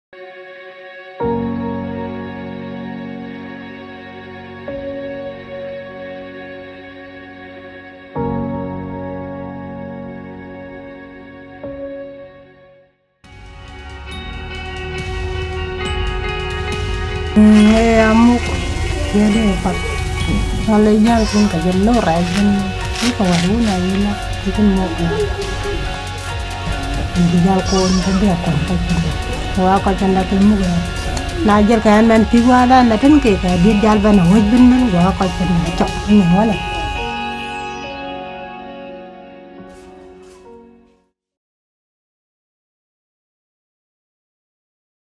Cuatro agentes de cambio en Sudán del Sur: Achoc
4 agentes de cambio en SudándelSur trabajan para combatir una crisis nutricional agravada por 4 años de conflicto: una voluntaria de Acción contra el Hambre, un doctor, una madre líder comunitaria y una emprendedora